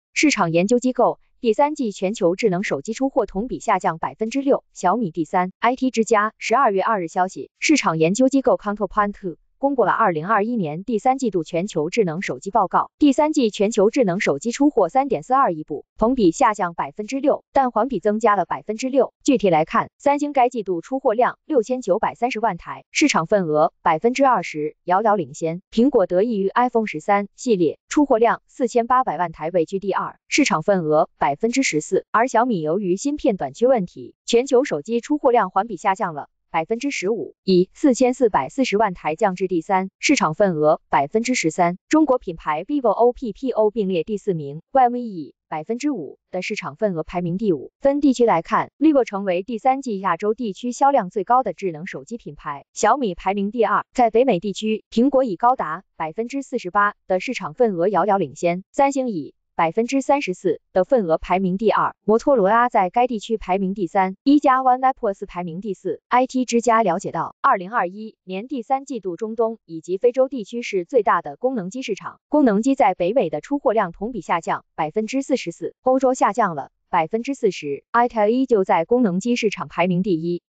市场研究机构：第三季全球智能手机出货同比下降6% 小米第三
IT之家 12月2日消息，市场研究机构Counterpoint公布了2021年第三季度全球智能手机报告。第三季全球智能手机出货3.42亿部，同比下降6%，但环比增加了6%。具体来看，三星该季度出货量 6930 万台，市场份额 20% 遥遥领先。苹果得益于 iPhone 13 系列，出货量 4800 万台位居第二，市场份额 14%。而小米由于芯片短缺问题，全球手机出货量环比下降了 15%，以 4440 万台降至第三，市场份额 13%。中国品牌 vivo、OPPO 并列第四名，realme 以 5% 的市场份额排名第五。分地区来看，vivo 成为第三季亚洲地区销量最高的智能手机品牌，小米排名第二。在北美地区，苹果以高达 48% 的市场份额遥遥领先，三星以 34% 的份额排名第二。摩托罗拉在该地区排名第三，一加 OnePlus 排名第四。IT之家了解到，2021 年第三季度中东以及非洲地区是最大的功能机市场。功能机在北美的出货量同比下降 44%，欧洲下降了 40%。itel 依旧在功能机市场排名第一。